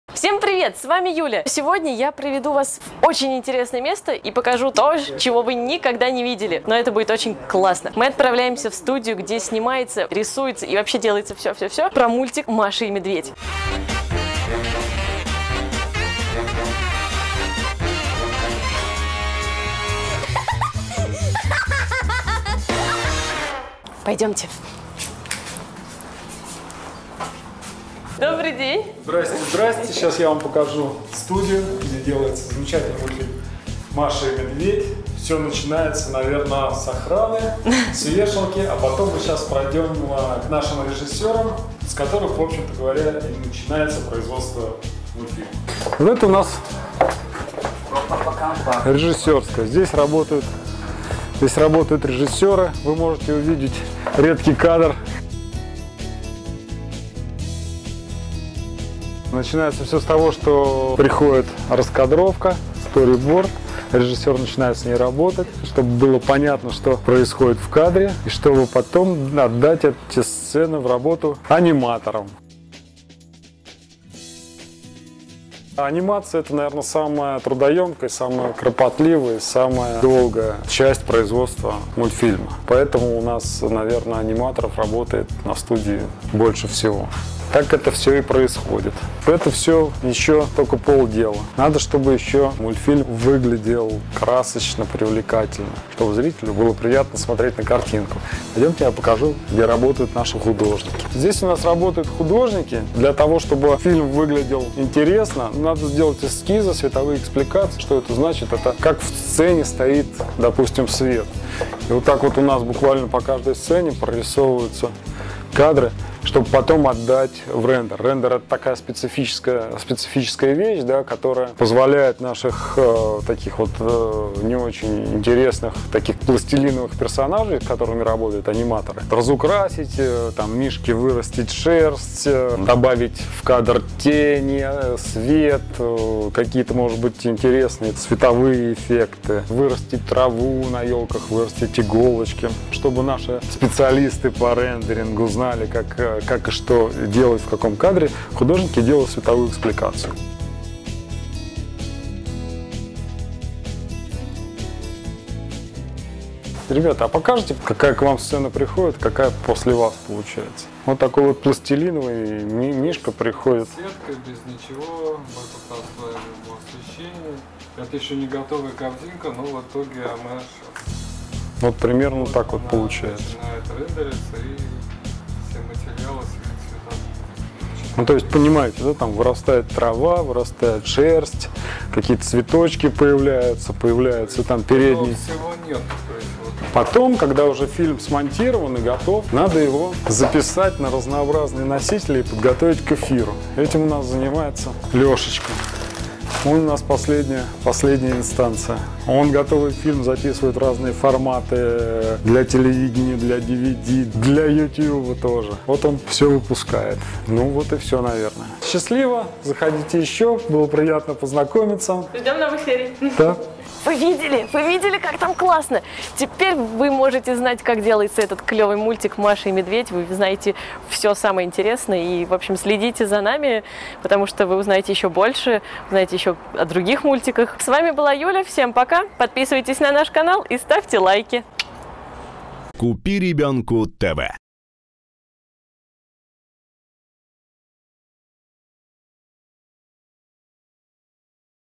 В гостях у Маши и Медведя (как создают мультик)
Не так давно мы выбрались в гости к «Маше и Медведю», а именно, в студию "Animaaccord". Там мы познакомились с создателями мультфильма, пообщались и прошлись по студии, где нам рассказали все о том, как создается каждая серия мультика от  первого до последнего кадра.
Смогли лицезреть тех, кто рисует, оживляет и наделяет героев мультика всеми их качествами. Увидели это сами и теперь делимся с вами!
Показываем все, что остается за кадром!

А мы будем прыгать от счастья метра на 3!